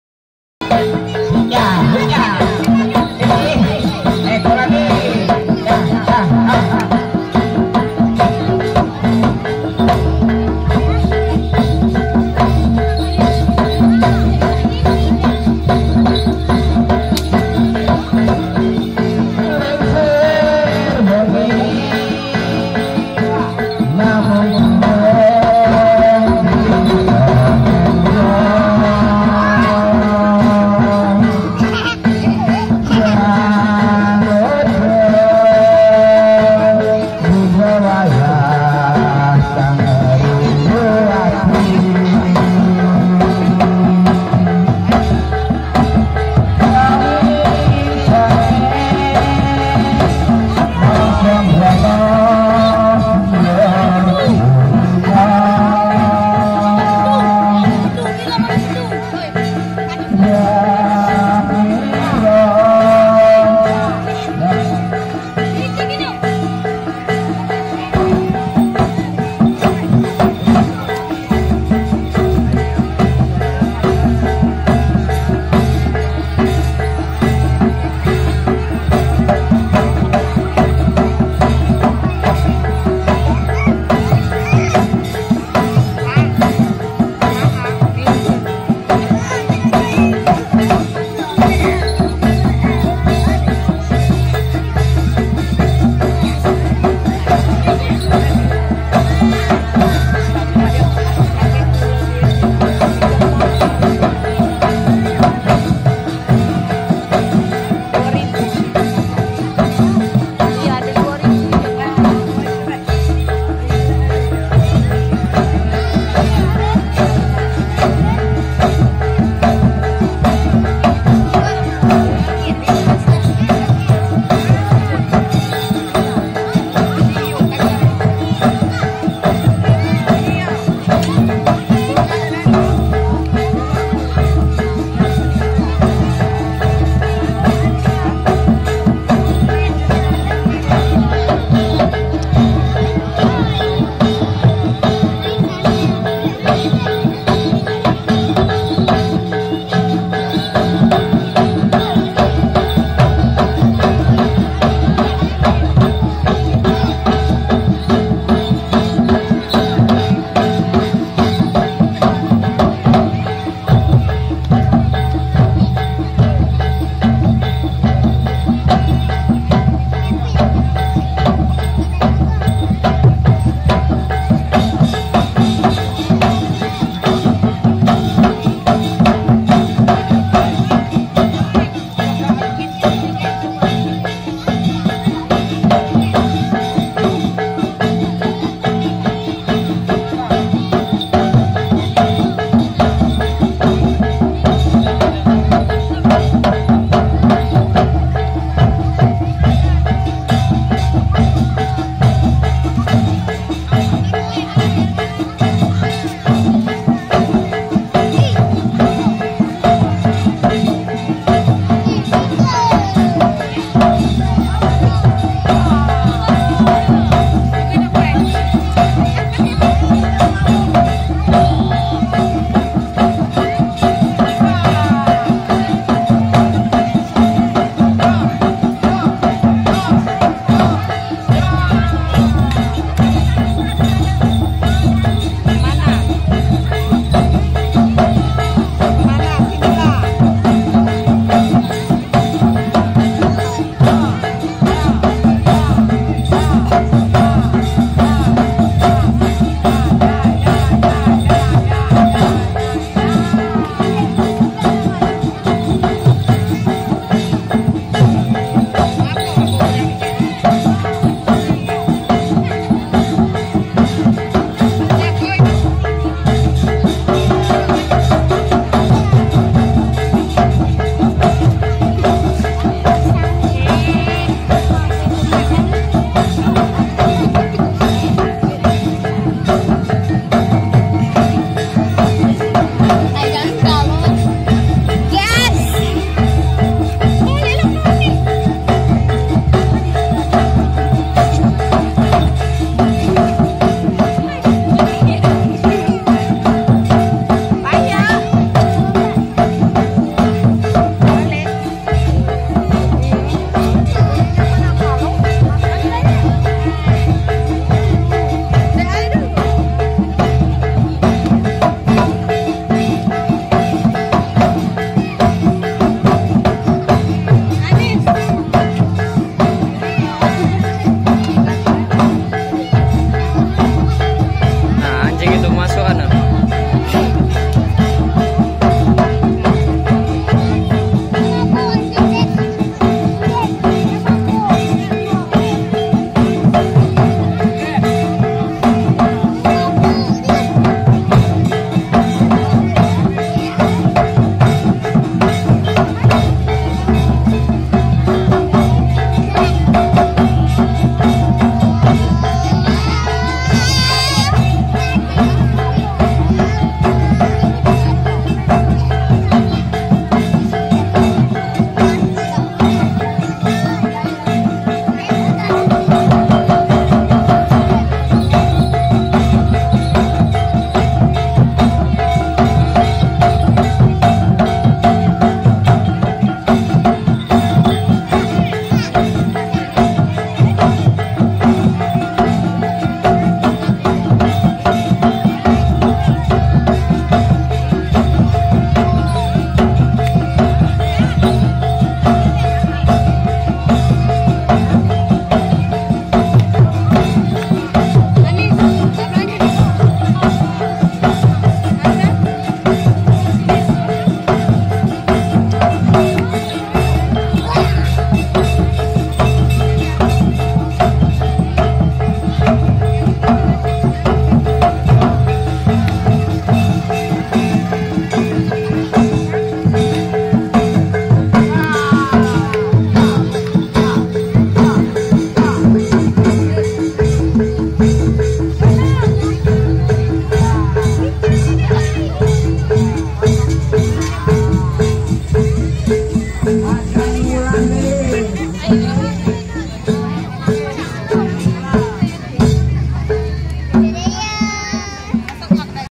Pentas Seni Kuda Lumping
Kuda lumping merupakan bagian dari seni yang dapat menghibur masyarakat